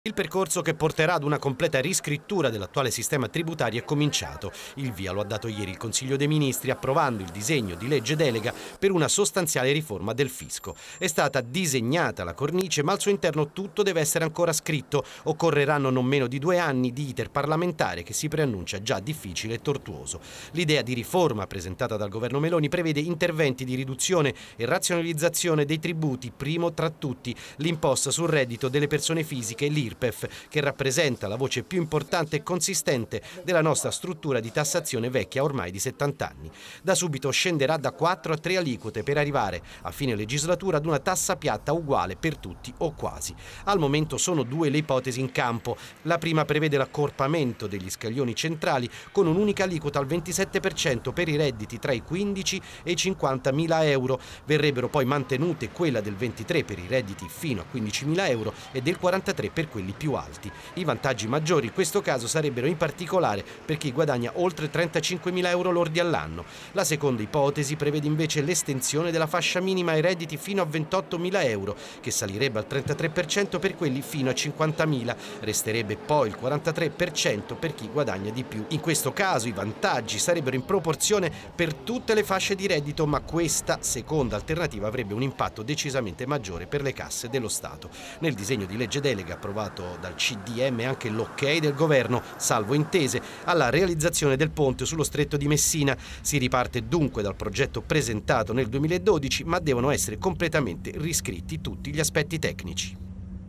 Riforma fiscale approvata, cosa cambia per i cittadini
Il governo ha dato il via libera alla delega fiscale, ma i decreti attuativi arriveranno entro 24 mesi. Cosa cambierà per i cittadini? Il servizio di Federico Plotti.